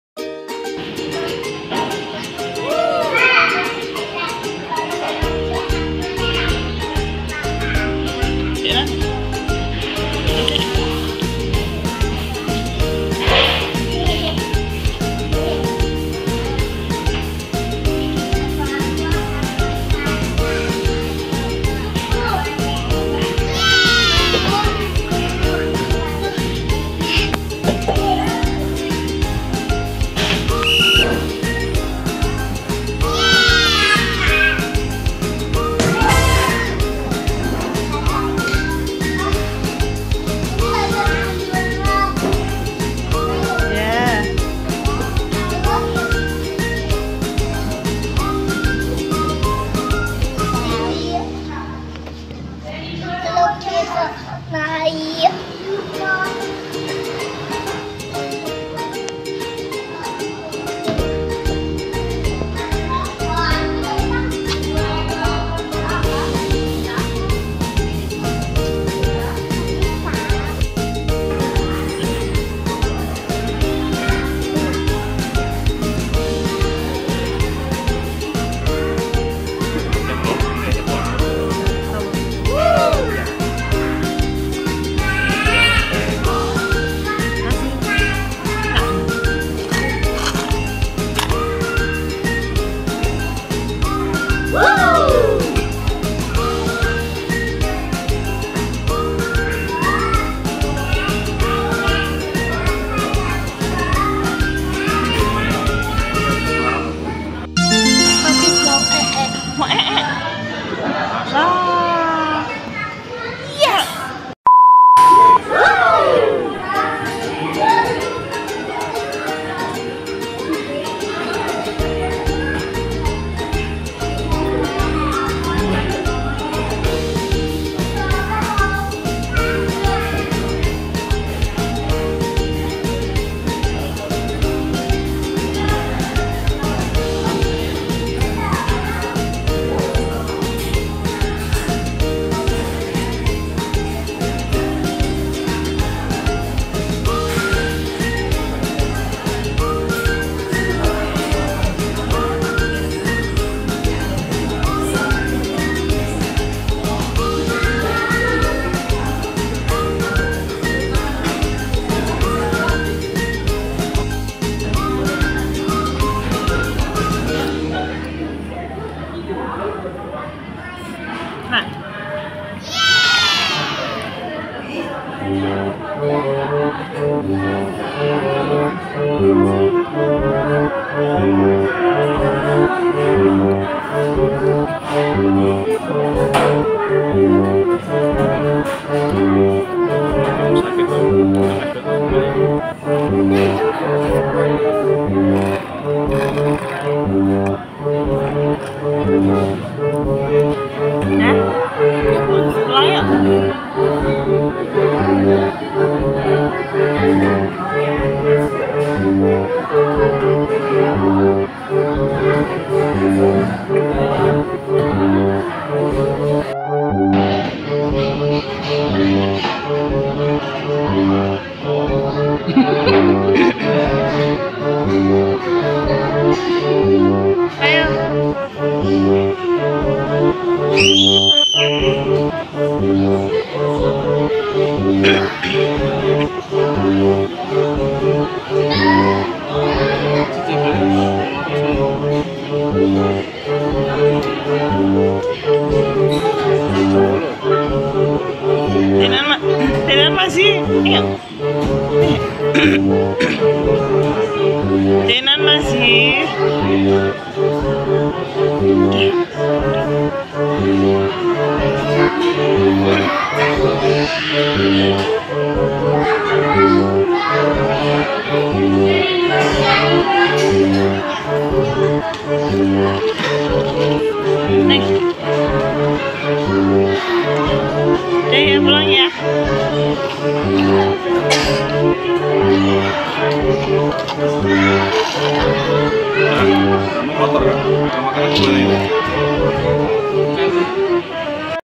Asyiknya Main Bersama 🤗
Hay Gaes .. kali ini si kembar main perosotan bersama temannya

Terimakasih
Salam Sayang 😘